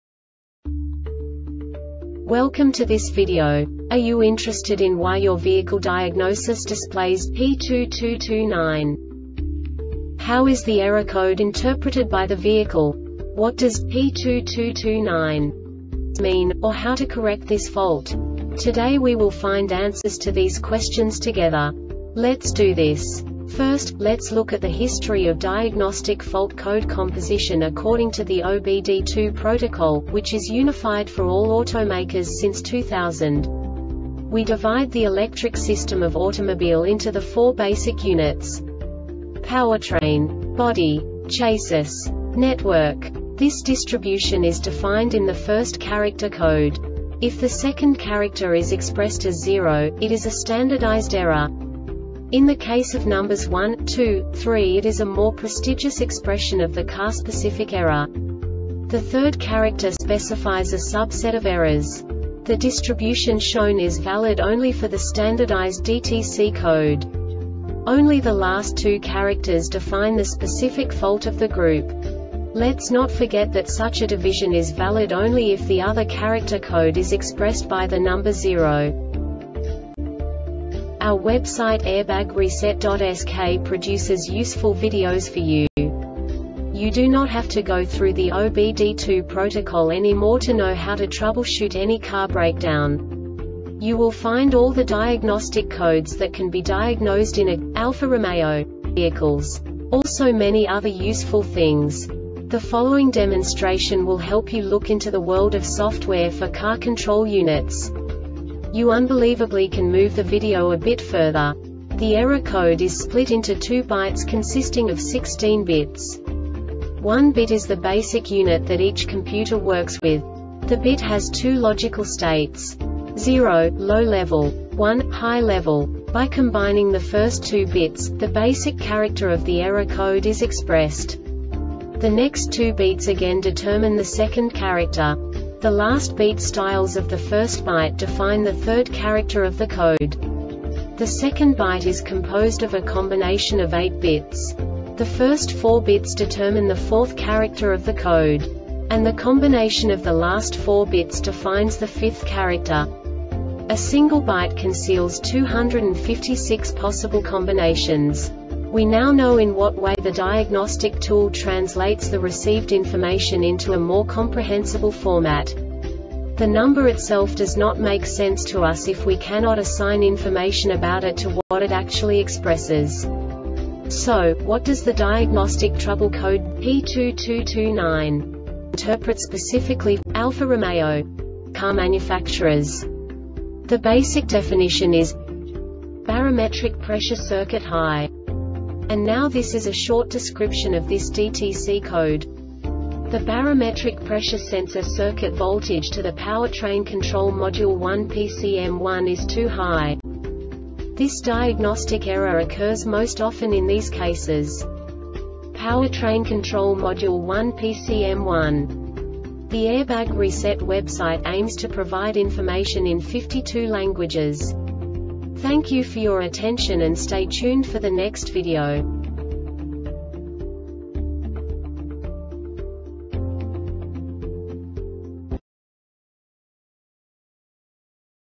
DTC Alfa-Romeo P2229 Short Explanation
Contents:
0:21 Basic DTC analysis according to OBD2 protocol standard.
1:48 Insight into programming
2:58 A diagnostically understandable description of the Diagnostic Trouble Code
3:05 Basic error definition

Make:
Alfa Romeo
Code:
P2229
Definition:
Barometric Pressure Circuit High
Description:
The Barometric Pressure Sensor circuit voltage to the Powertrain Control Module 1 (PCM-1) is too high.
Cause:
Powertrain Control Module 1 (Pcm-1)